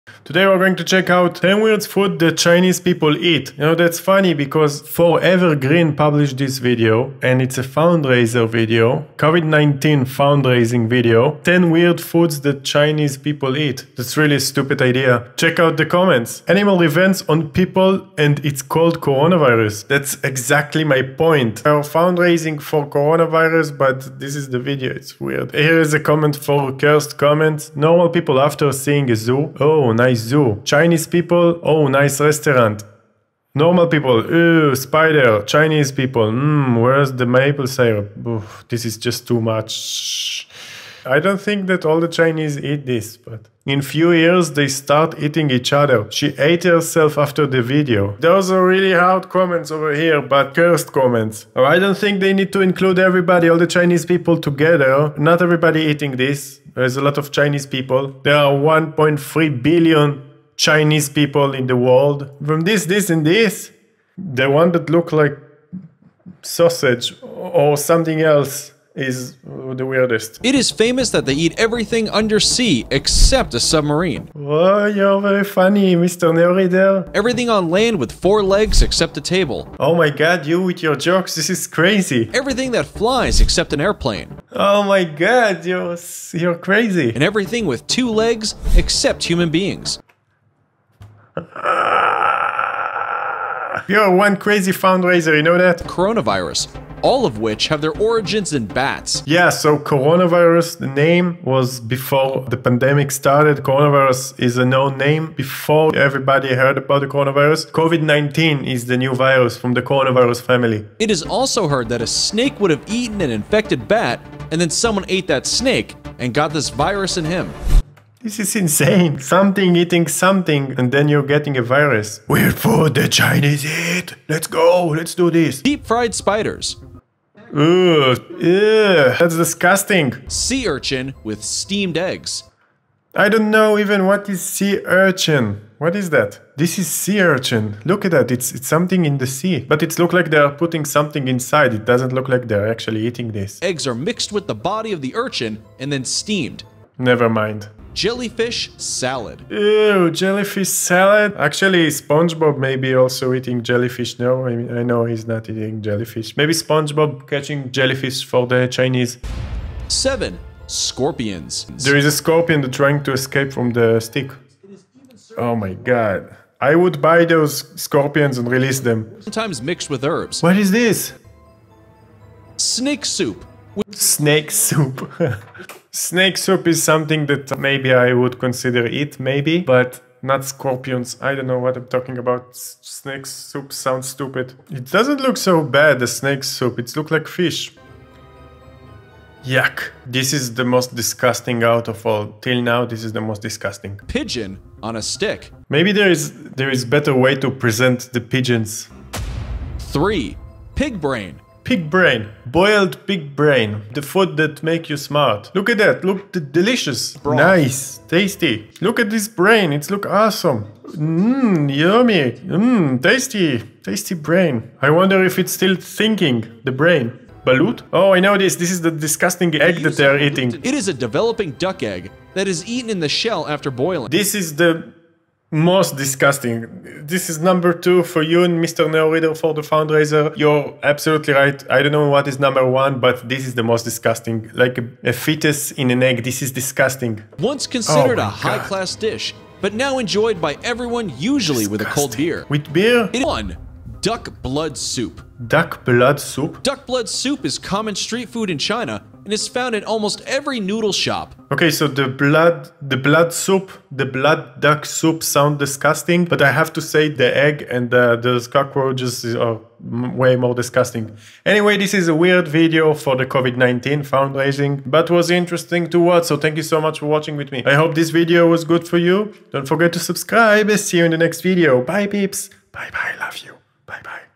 Weird Food in China
Check out weird food that people eat in CHINA!
The Chinese are well known when it comes to eat weird things. It is famous that they eat everything under sea except a submarine! Everything on land with four legs except a table, everything that flies except an airplane and everything with two legs except human beings!

Have a video for review? Write me a comment, promise i'll try to make a video about it!